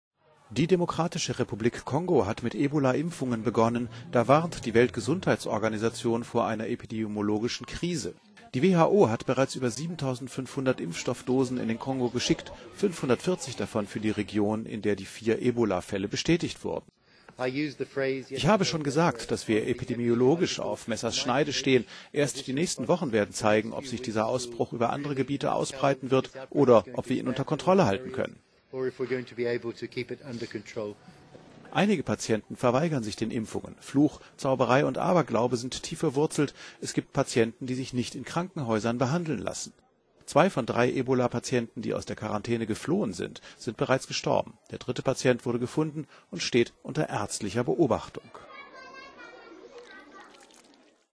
WHO: Ebolaausbruch "auf Messers Schneide"
Impfungen haben begonnen, aber erst die nächsten Wochen werden zeigen, ob im Kongo eine Epidemie droht oder ob die Infektion unter Kontrolle gebracht werden kann.…